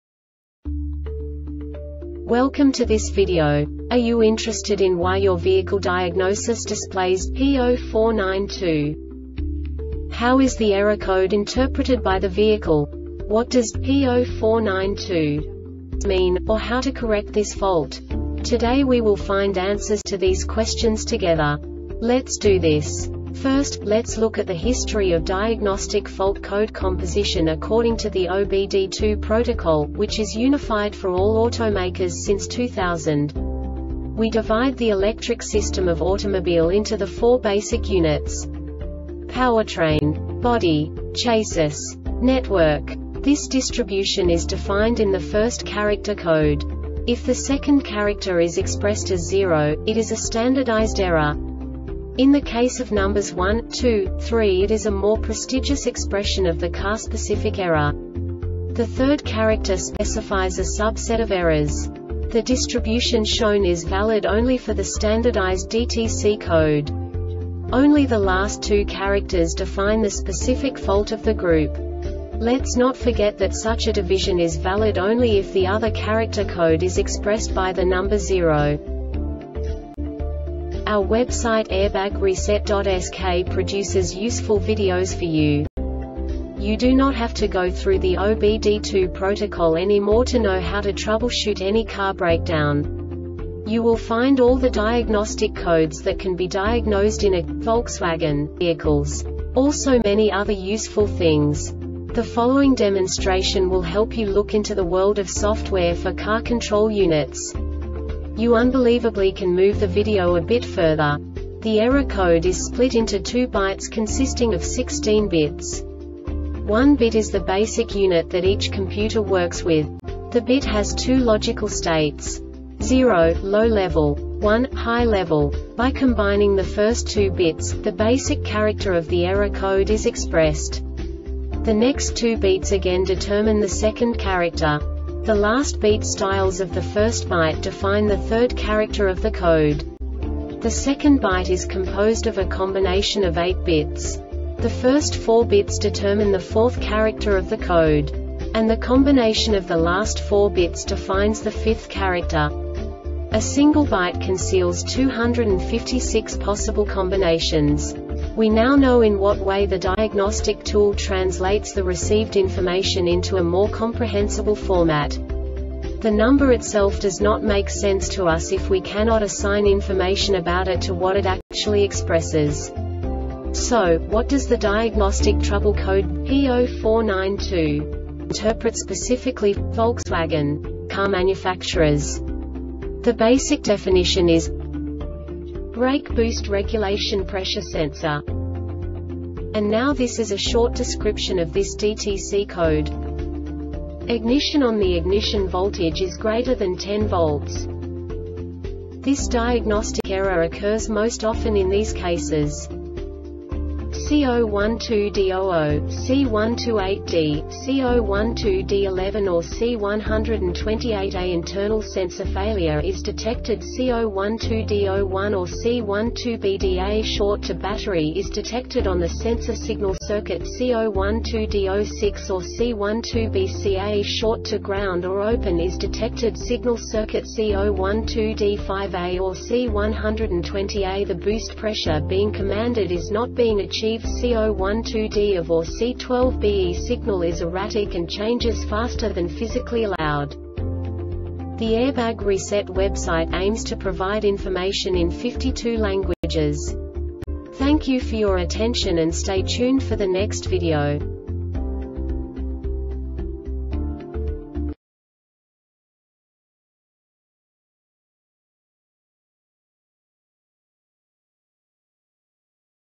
DTC Volkswagen P0492 Short Explanation
Contents:
0:21 Basic DTC analysis according to OBD2 protocol standard.
1:48 Insight into programming
2:58 A diagnostically understandable description of the Diagnostic Trouble Code
3:05 Basic error definition

Make:
Volkswagen
Code:
P0492
Definition:
Brake Boost Regulation Pressure Sensor
Description:
Ignition ON.The ignition voltage is greater than 10 V.
Cause:
C012D 00, C128D,C012D 11 or C128AInternal sensor failure is detected.C012D 01 or C12BDA short to battery is detected on the sensor signal circuit.C012D 06 or C12BCA short to ground or open is detected signal circuit.C012D 5A or C120AThe boost pressure being commanded is not being achieved.C012D OF or C12BESignal is erratic and changes faster than physically allowed.